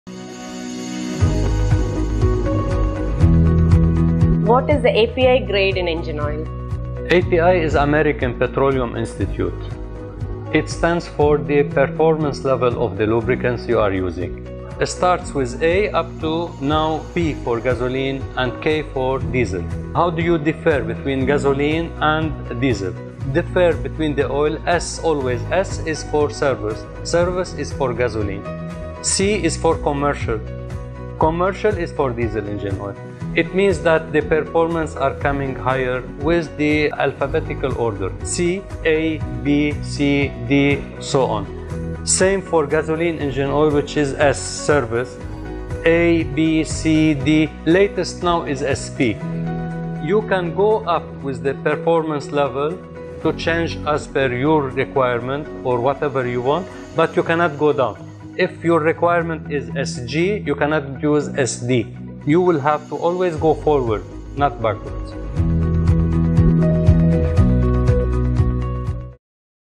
What is the API standard for oil?
Even if you are new to automobile maintenance, chances are, you might have already heard about API engine grades. Most people know what engine oil grades are but lack the knowledge regarding where these grades come from, how these grades are measured, and who is responsible for setting these universal grades. In a nutshell, API is responsible for engine grades. The service rating of passenger cars and commercial automotive motor oils is classified by the American Petroleum Institute (API). The program certifies that an oil meets certain Original Equipment Manufacturer (OEM) quality and performance standards.